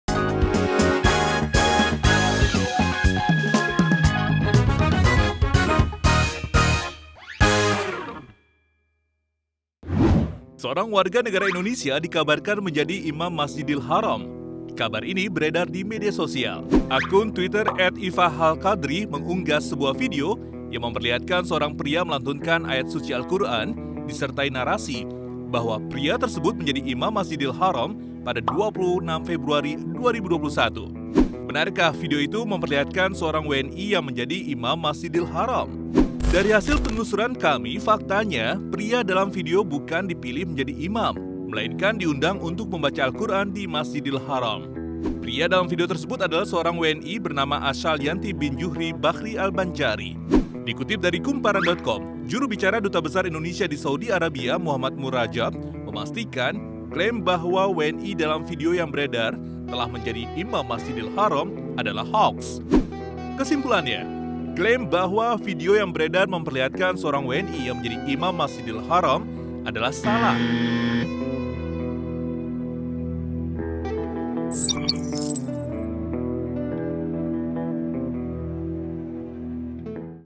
[Ceki-ceki] WNI Jadi Imam Masjidil Haram? Simak Faktanya
Seorang WNI dikabarkan menjadi Imam Masjidil Haram. Kabar ini beredar di media sosial pada akun twitter @iffahAlqadrie menggunggah sebuah video yang memperlihatkan seorang pria melantunkan ayat suci Al-Qur'an disertai narasi bahwa pria tersebut menjadi Imam Masjidil Haram pada 26 Februari 2021.

Benarkah informasi tersebut? Yuk, simak faktanya berikut ini!

Dapatkan berita-berita terpercaya lainnya di @cekfaktamedcom